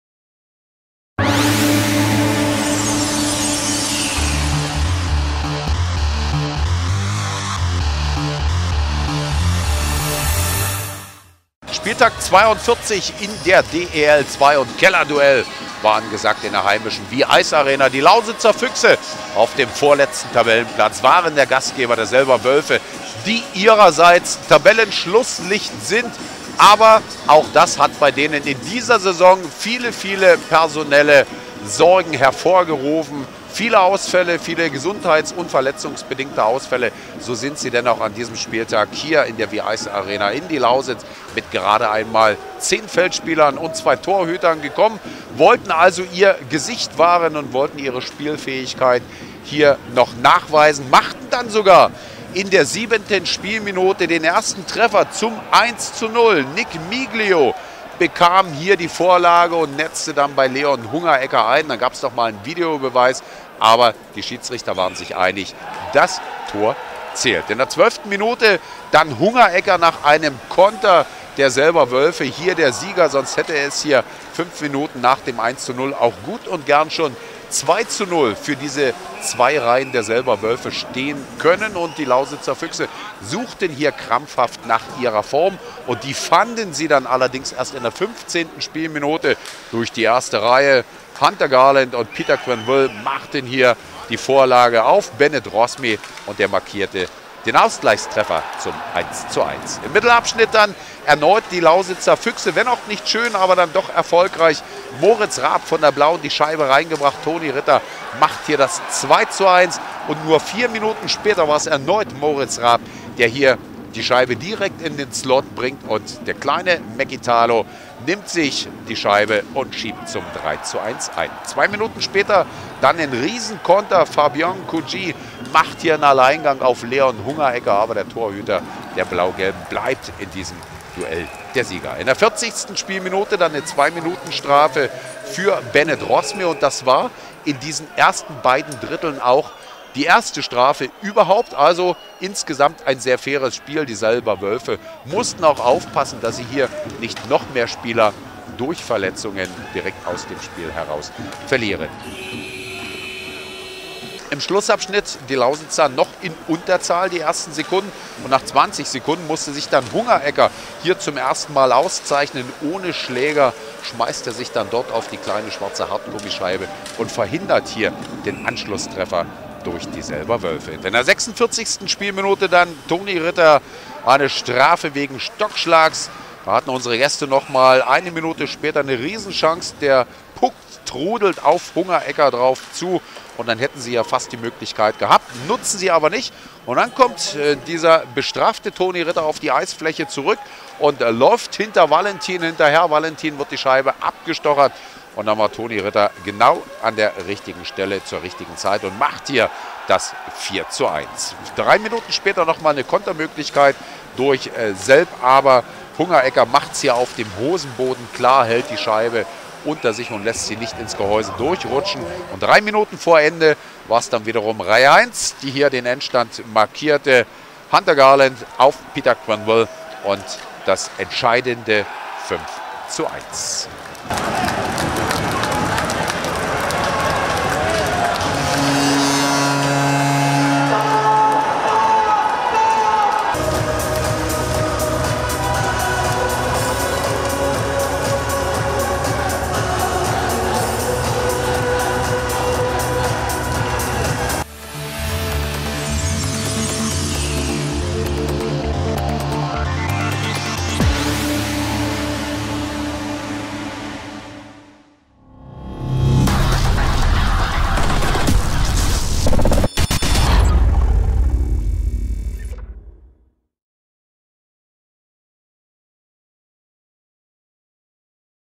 42. Spieltag - DEL2 Saison 2021/2022 | Lausitzer Füchse vs. Selber Wölfe | 5:1 (ZF)
Zusammenfassung des Spiels der Lausitzer Füchse gegen den Aufsteiger aus Selb. Die Füchse gewannen gegen eine stark dezimierte, aber tapfer kämpfende Selber Mannschaft mit 5:1.